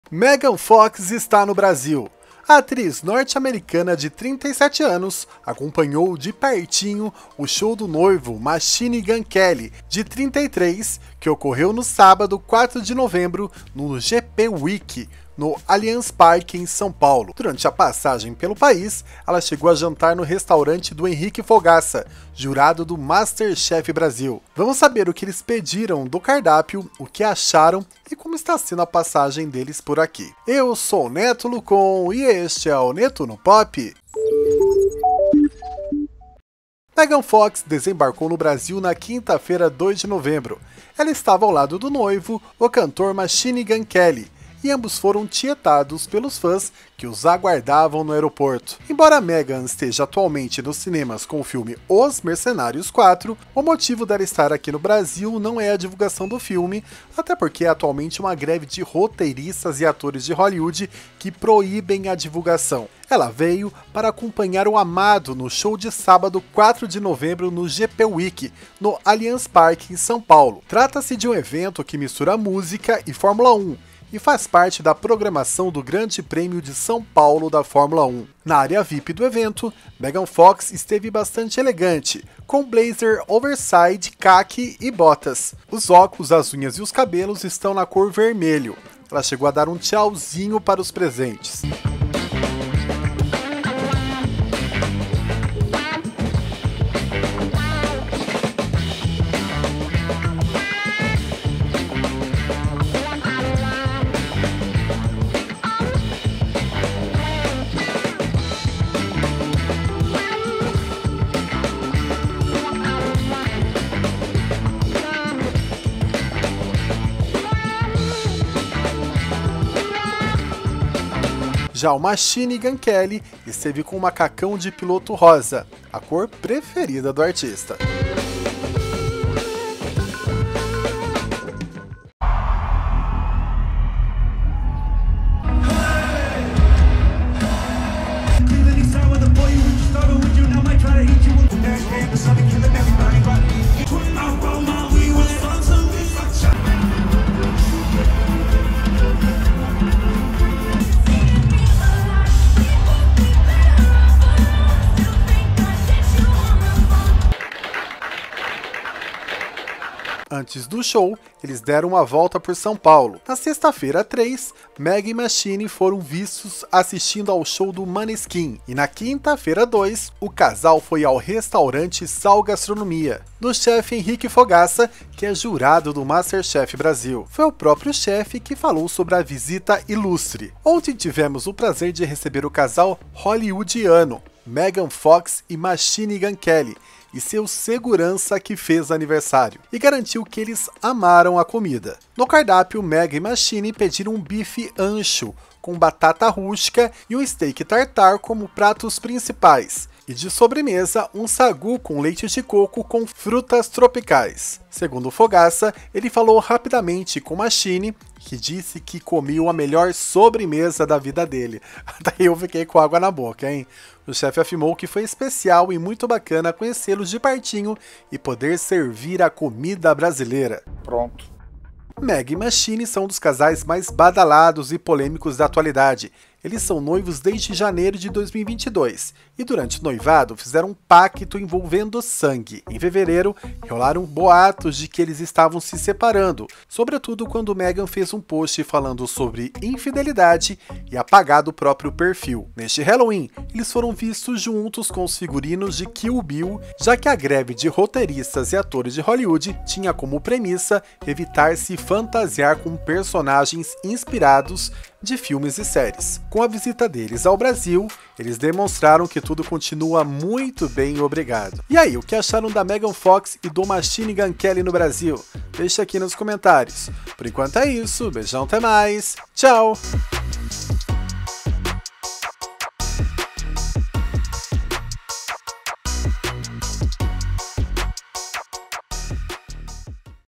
Megan Fox no Brasil | Atriz assiste show do noivo Machine Gun Kelly e prova comida do chef Fogaça
Megan Fox está no Brasil. A atriz norte-americana de 37 anos acompanhou de pertinho o show do noivo Machine Gun Kelly, de 33, que ocorreu no sábado, 4 de novembro, no GP Week, em São Paulo.

Durante a passagem, ela chegou a jantar no restaurante do Henrique Fogaça, jurado do Master Chef. Vamos saber o que ela pediu, o que achou e como está sendo a passagem dela por aqui.

Fotos tumb: Van Campos/Agnews